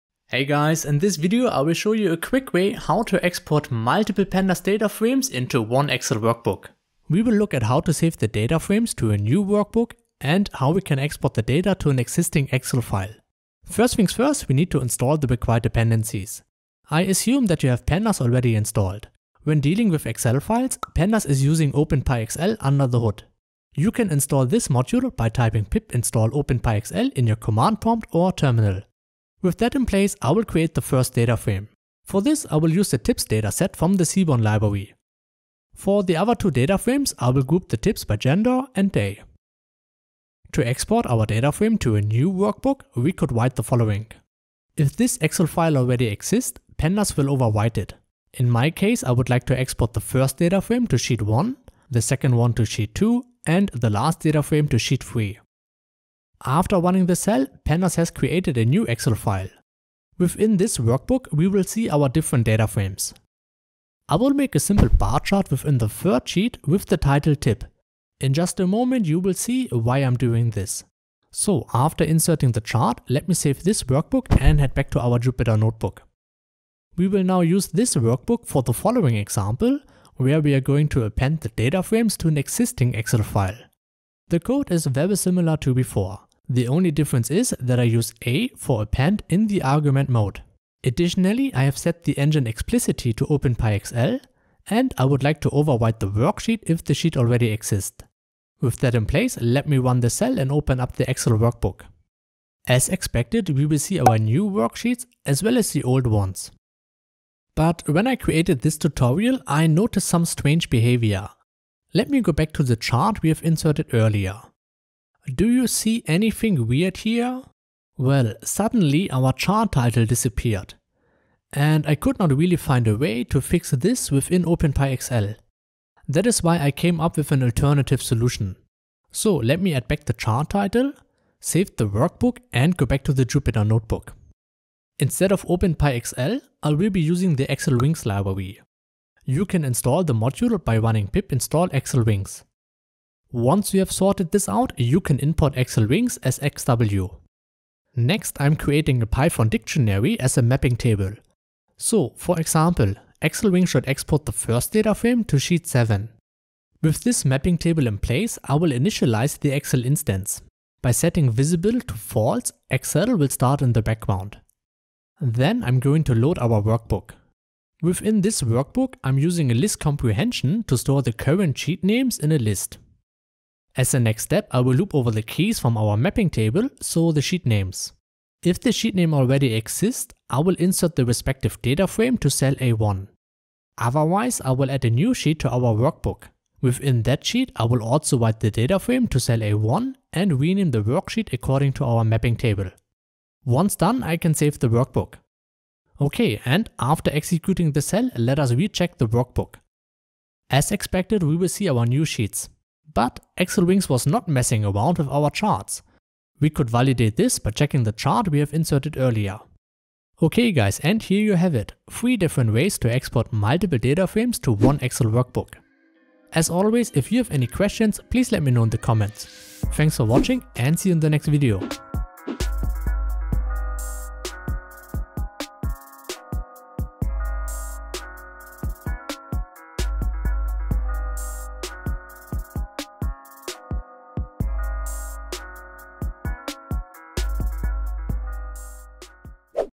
Export Pandas DataFrames to new & existing Excel workbook
𝗗𝗘𝗦𝗖𝗥𝗜𝗣𝗧𝗜𝗢𝗡
In this tutorial, I'll show you how to export a Pandas dataframe to a new Excel workbook, or to an existing Excel workbook. Learn how to save your data in an easy-to-use format that you can take anywhere.